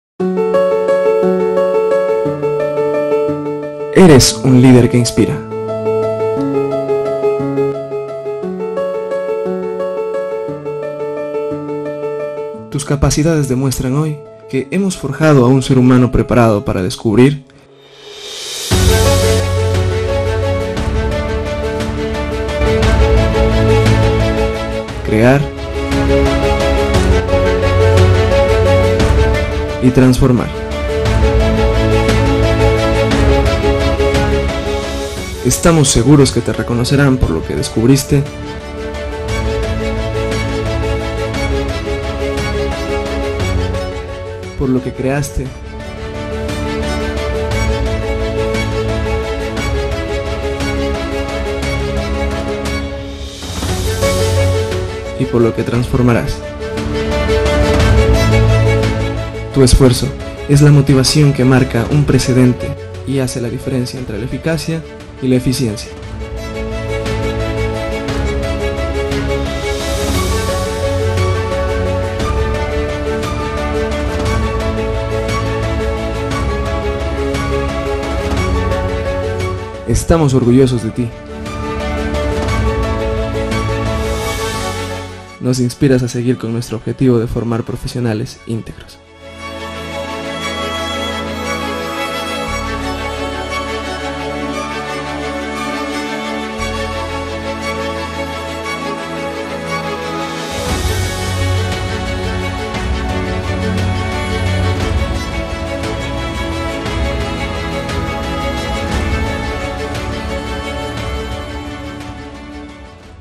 ¿Cómo es el mejor graduado de la PUCE Santo Domingo?
Eres un líder que inspira

-Excelencia Académica
- Investigación Científica
- Certificaciones Internacionales en inglés y Competencias Digitales
- Trabajo de titulación con impacto en la sociedad
- Competencias actitudinales

Tu esfuerzo es una motivación que marca un precedente y hace la diferencia entre la eficacia y la eficiencia.

Nos inspiras a seguir con nuestro objetivo de formar profesionales íntegros.

Estamos orgullosos de ti.